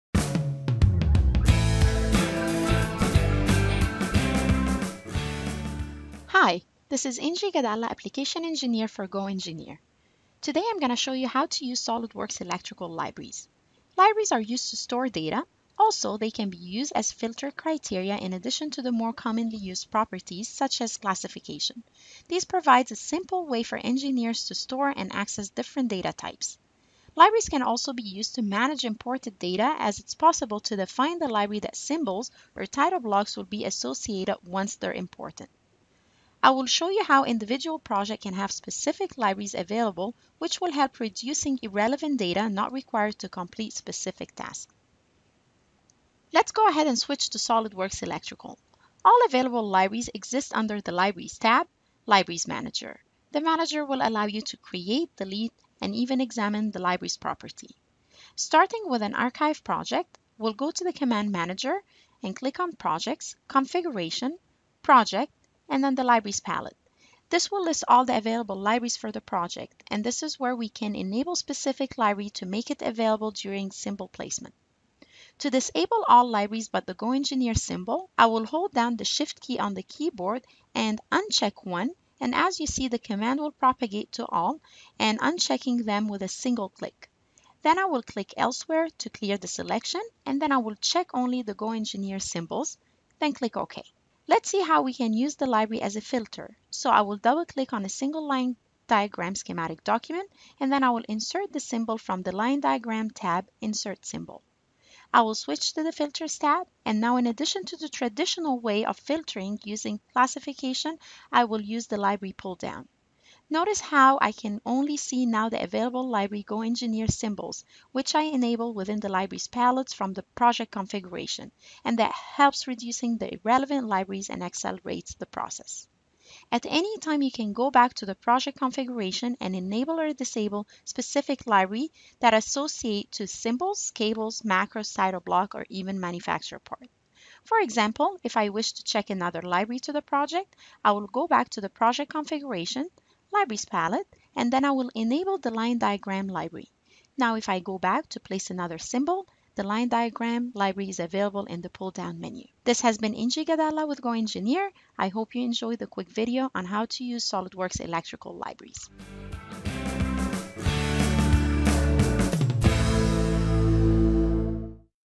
SOLIDWORKS Electrical Quick Tip - Using Electrical Libraries
Learn how to use SOLIDWORKS Electrical libraries and how individual projects can have specific libraries available.
SOLIDWORKS Quick Tip presented by Injy Gadalla of GoEngineer.